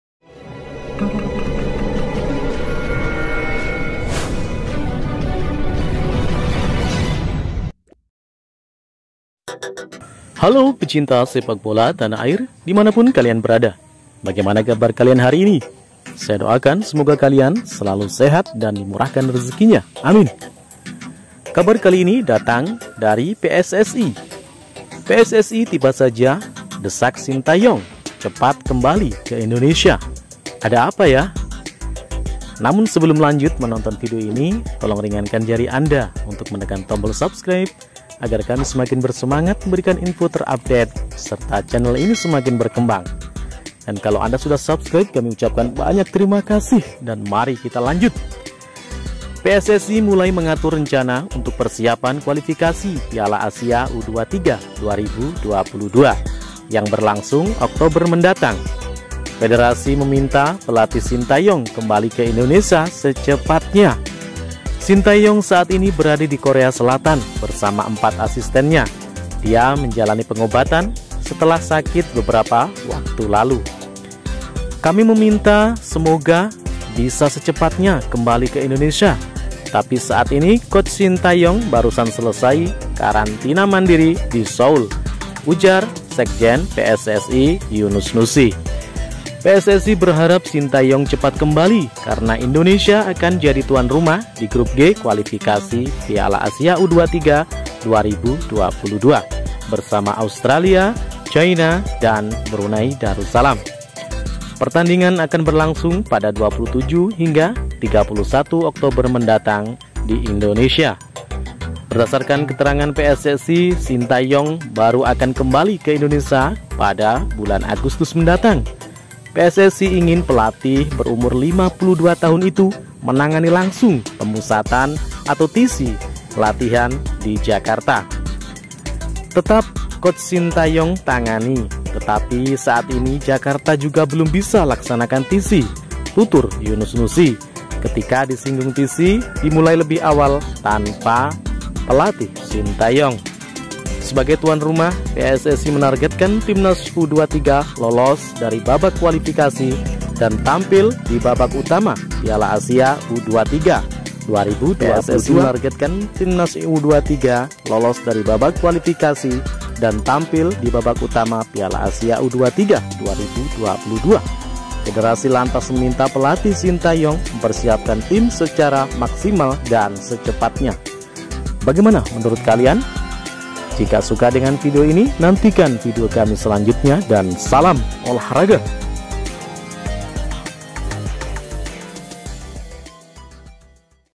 PSSI, desak shin tae yong Balik ke Indonesia secepatnya//sedarurat itu kah, Ada apa?
jadi tuan rumah Kualifikasi piala AFC 2022,pssi desak shin tae yong pulang ke Indonesia secepatnya mungkin#timns Indonesia#shin tae yong#pssi#yunus nusi